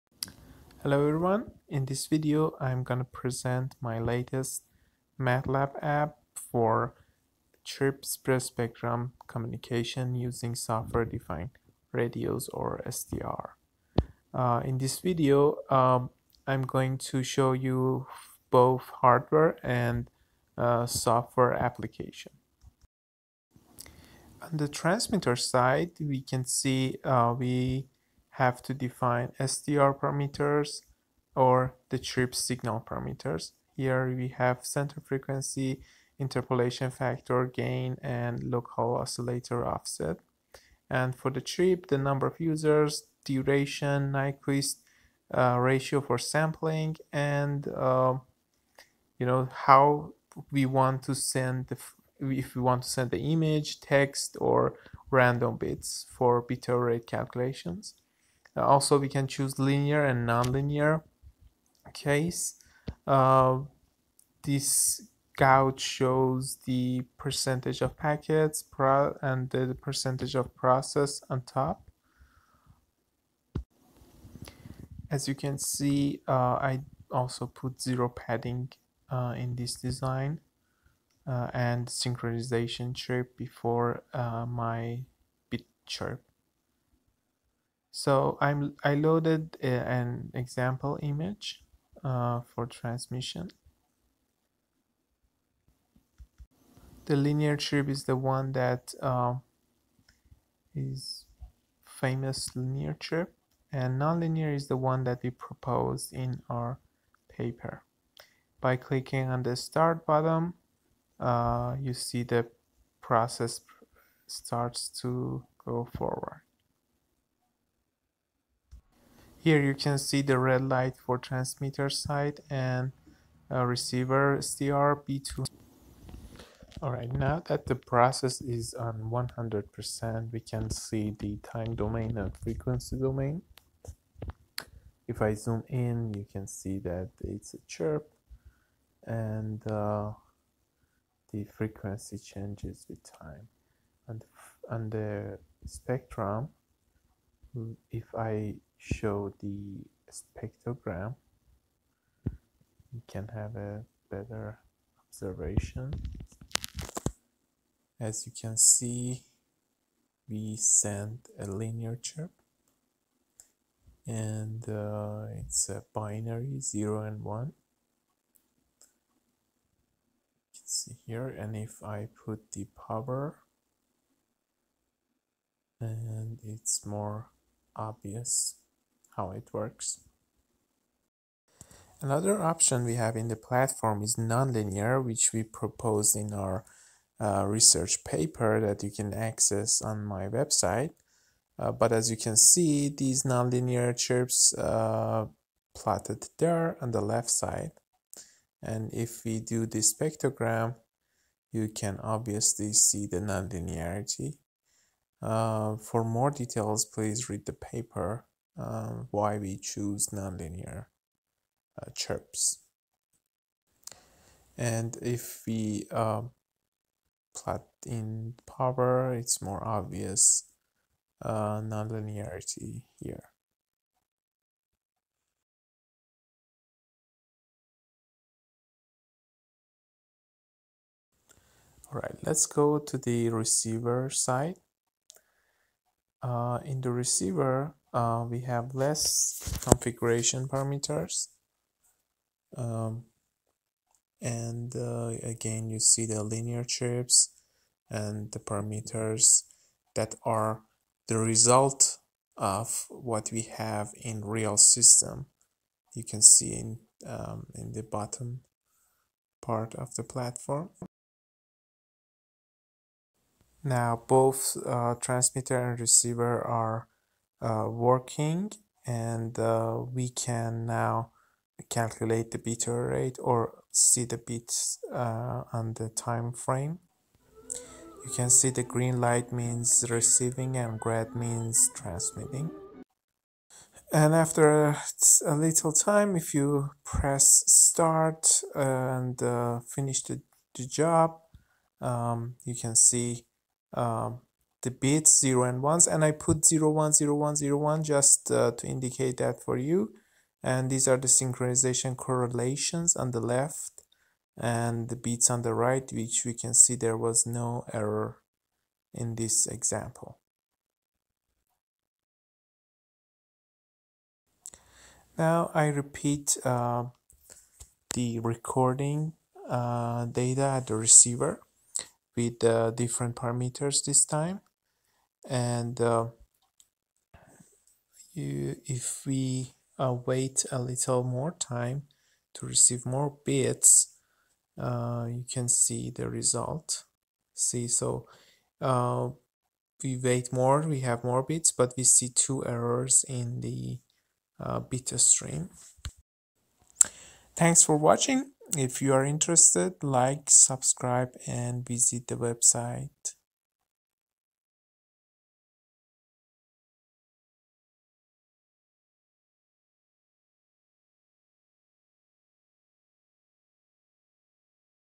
Chirp Spread Spectrum Transmission using Software Defined Radios (SDR) in MATLAB App Designer
In this video, I show my latest MATLAB app designer for CSS transmission using SDR.